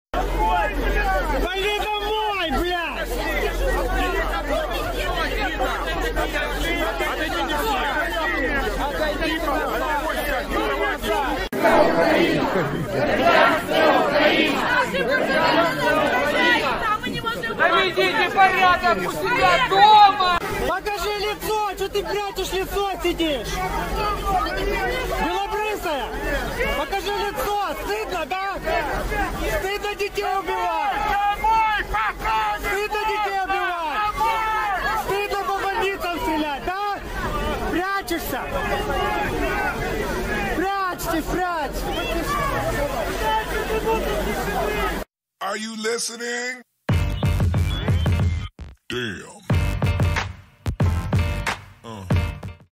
Citizens of BERDYANSK (Ukraine) protest RUSSIAN occupation.
Citizens drove those Russians away from there main square. Ukrainassa sijaitsevan Berdyanskin asukkaat protestoivat venäläisiä miehittäjiä vastaan. Ajavat lopulta joukot pois torilta. Rohkeaa tekemistä ukrainalaisilta.